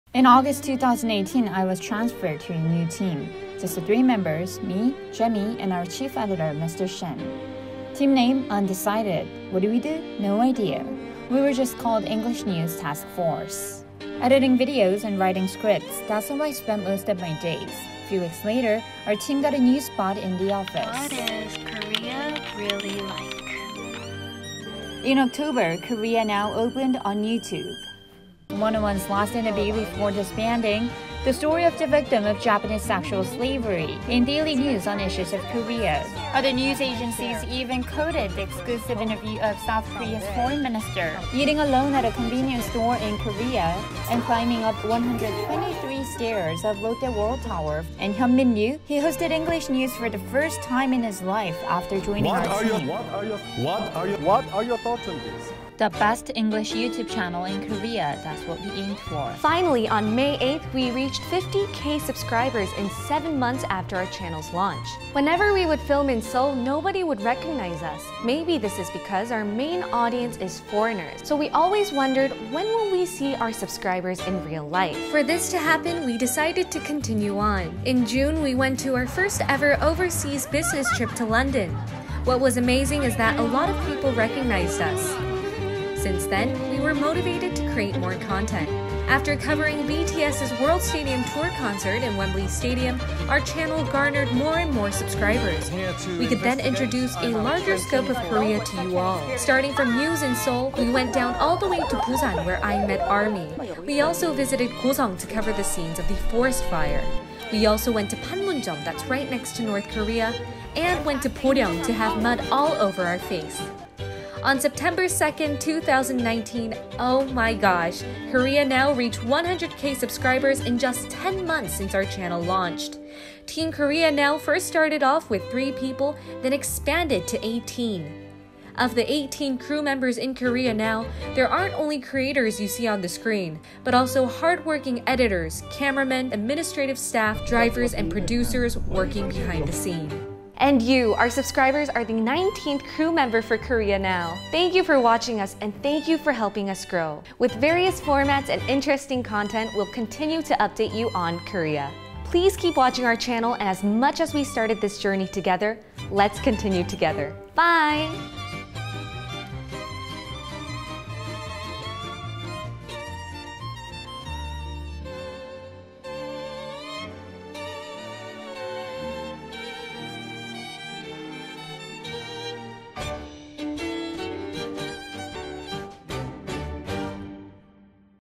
KOREA NOW makes history, with subscribers all around world, memorating 100k!!
KOREA NOW
English YouTube news channel KOREA NOW has its own website.🎊🥰🤩
Yonhap News, a Korean national news agency, has launched a koreanow.com, with the reach of 100,000 subscribers.
On September 9th (Korean date), we celebrated the official establishment of our website at the Yohap News Agency headquarters in Seoul. 🇰🇷
With such advancement, KOREA NOW will become widely available to more subscribers around the world with the two-track Youtube/Website system.📱🖥
We promise to provide you with high quality and interesting content about Korea.
A short clip commemorating the 100,000 subscribers will be uploaded on September 10th (Korean date). Please look forward to it.

We sincerely appreciate your support.🤗

Ji Hong Shin, Editor of KOREA NOW